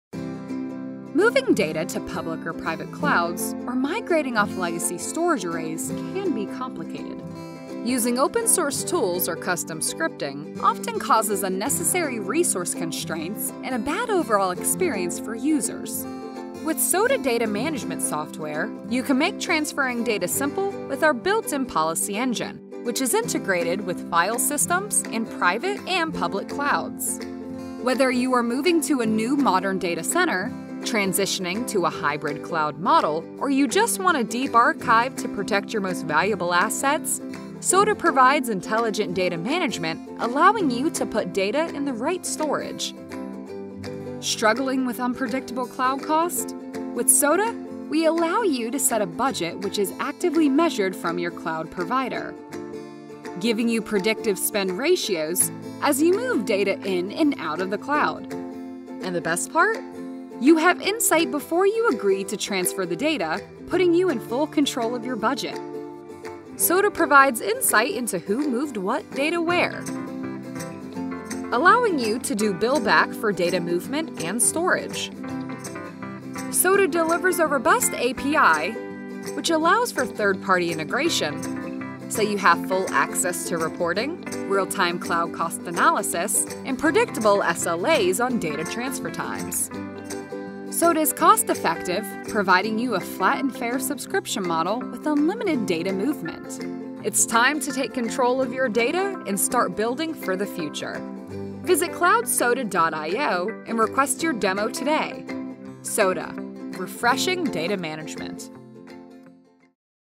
CloudSoda: Refreshing Data Management
It's time to take control of your budget and how you manage your data to and from the cloud. The explosion of data in your environment can become overwhelming and having a simple way to manage its whereabouts is important. CloudSoda helps deliver smarter data movement while providing valuable insights into your total cost between on-premise and cloud. Want to learn more? Request a demo